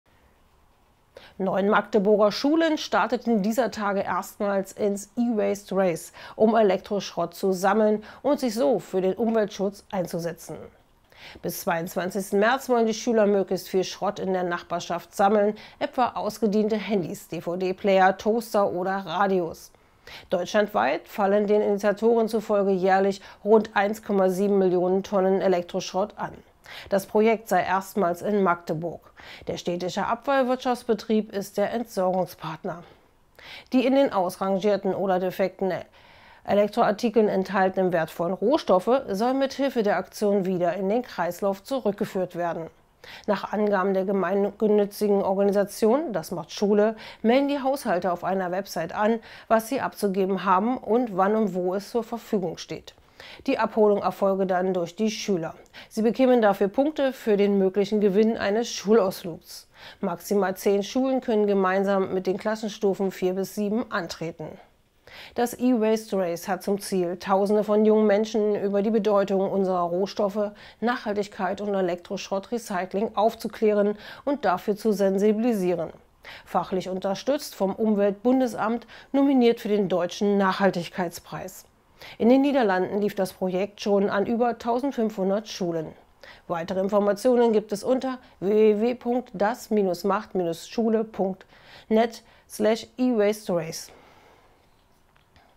Schulumweltprojekt E-Waste Race startet erstmals
9 Magdeburger Schulen starteten dieser Tage erstmals ins E-Waste Race, um Elektroschrott zu sammeln und sich so für den Umweltschutz einzusetzen.
Bis  22. März wollen die Schüler möglichst viel Schrott in der Nachbarschaft sammeln, etwa ausgediente Handys, DVD-Player, Toaster oder Radios. Deutschlandweit fallen den Initiatoren zufolge jährlich rund 1,7 Millionen Tonnen Elektroschrott an. Das Projekt sei erstmals in Magdeburg. Der Städtische Abfallwirtschaftsbetrieb ist der Entsorgungspartner.